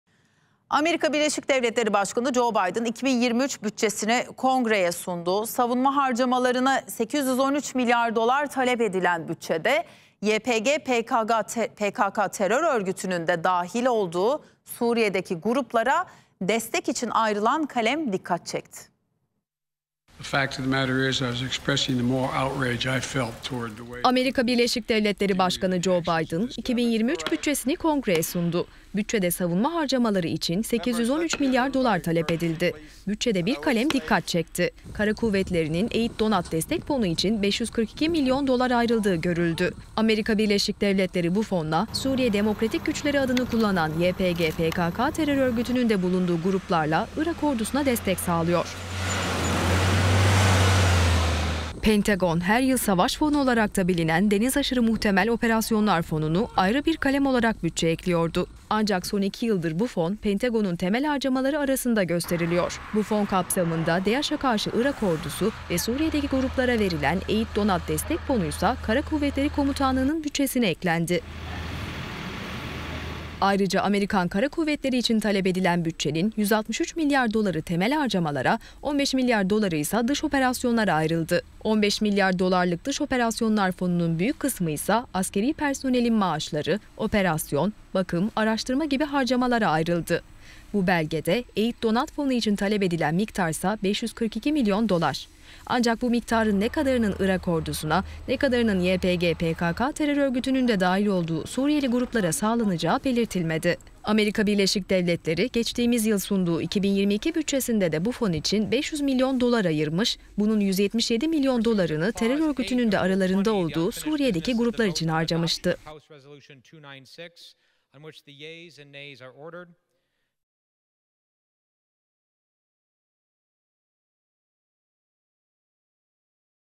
Biden'dan Skandal Hamle! Türkiye'yi Ukrayna Konusunda Överken Bunu Yaptı
Amerika Birleşik Devletleri (ABD) Kara Kuvvetlerinin 2023 mali yılı bütçesinde, aralarında SDG ismini kullanan YPG/PKK’nın da bulunduğu Suriye’deki gruplara ve Irak ordusuna 542 milyon dolar ayrıldı.